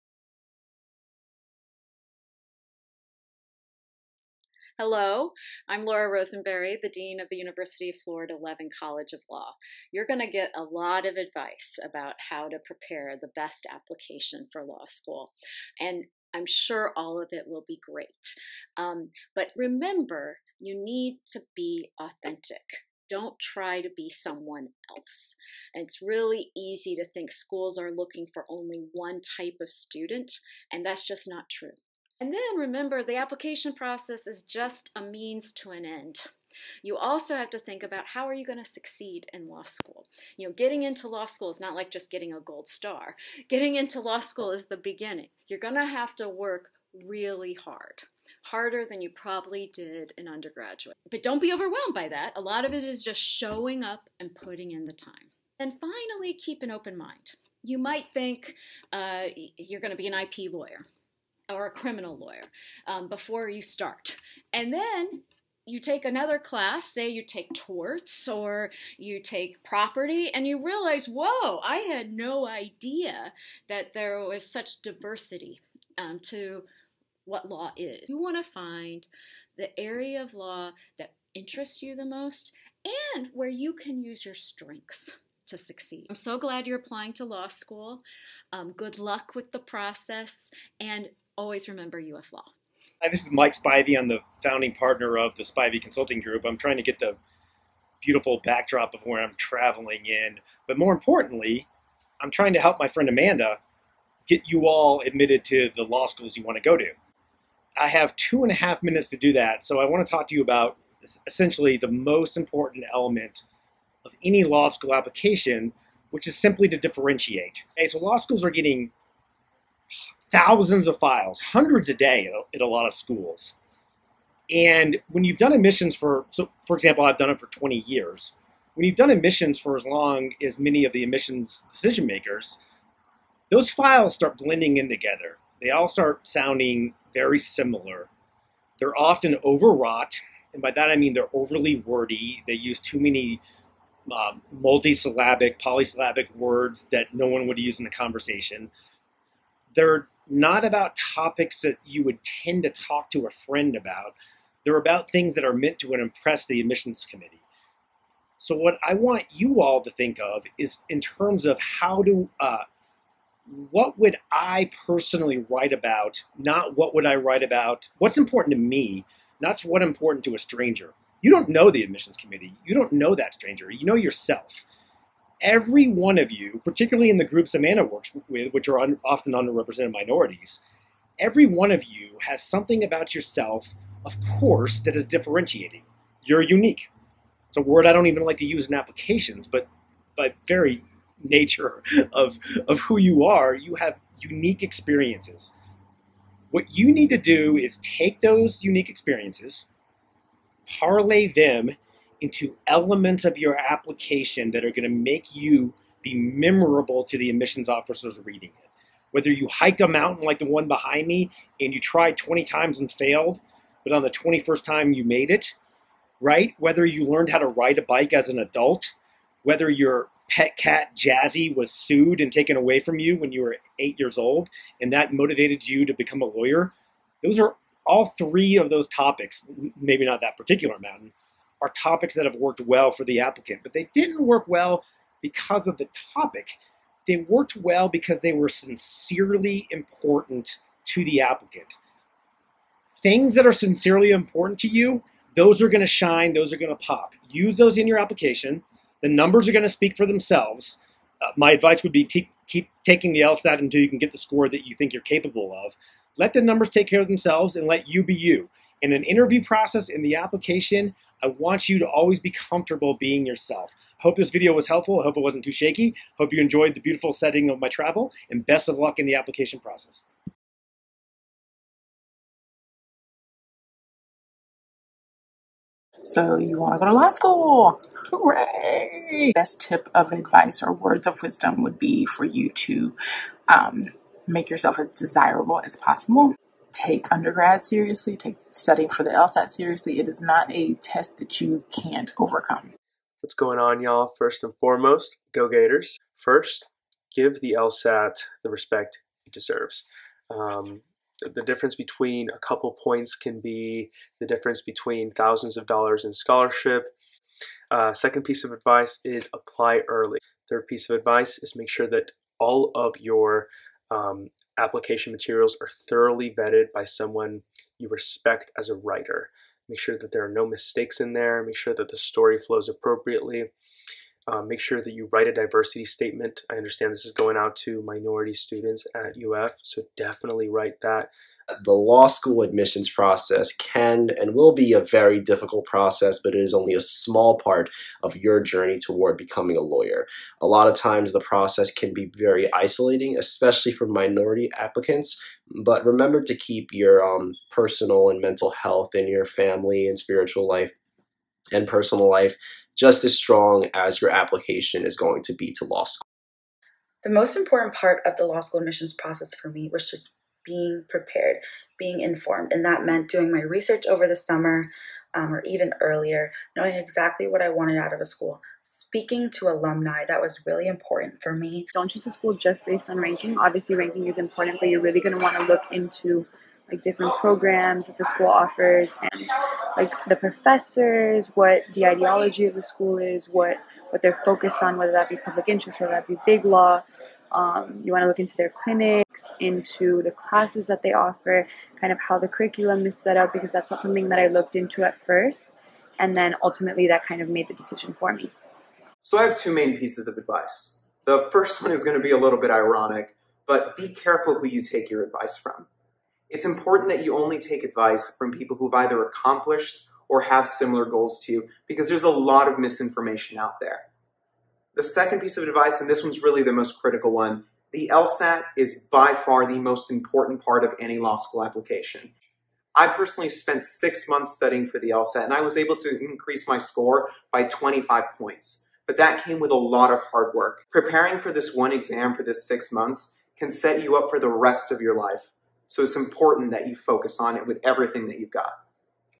How To Get Into Law School, featuring Dean of UF Law, Spivey Consulting, and minority law students
So, you want to go to law school? This video features admissions advice from experts and minority applicants who successfully earned admission to top programs. The video is intended not only to provide helpful insights, but also to remind minority applicants that there is a huge community ready and excited to help them through their journey. Good luck!